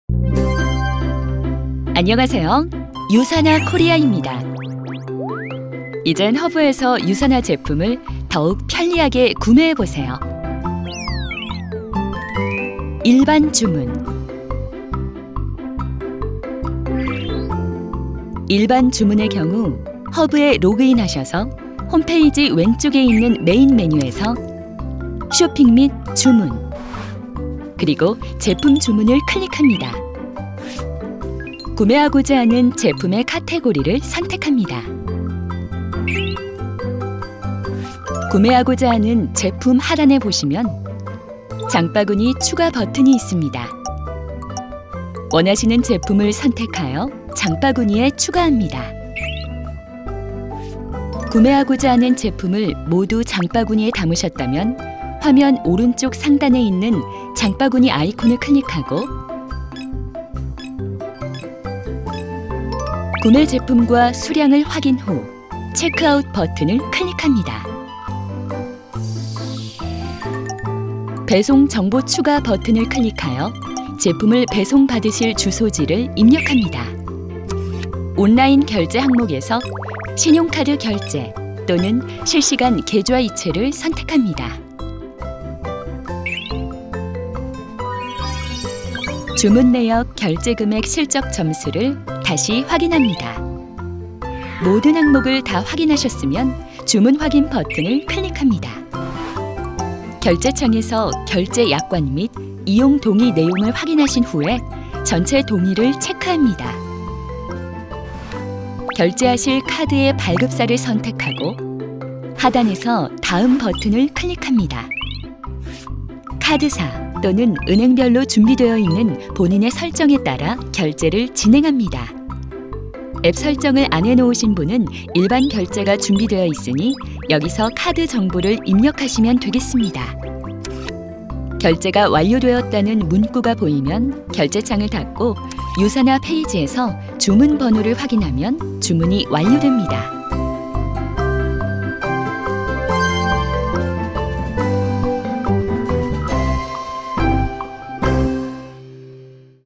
제품 구매할 때도 디지털로 편리하게 – 유사나 허브 일반주문 방법
편리한 디지털 시대! 💻💻💻
유사나 제품 허브에서 간편하게 구매해 보세요!

유사나 허브 일반주문 방법! ☝️

✔️1. 허브에 로그인 후 홈페이지 왼쪽 메뉴에서
'쇼핑 및 주문'과 '제품 주문'을 차례로 선택합니다

✔️2. 구매하고자 하는 제품의 카테고리 선택 후
원하시는 제품을 골라 '장바구니'에 추가합니다

✔️3. 장바구니에서 구매하실 제품과 수량 확인 후
'체크아웃'을 선택합니다

✔️4. '배송정보 추가'를 선택해 배송 받을 주소를 입력합니다

✔️5. '온라인 결제' 항목에서 결제 방법을 선택합니다

모든 항목을 확인 후 '주문 확인' 선택하면 끝!
허브로 제품 구매하기 참 쉽죠? 🤭🤭🤭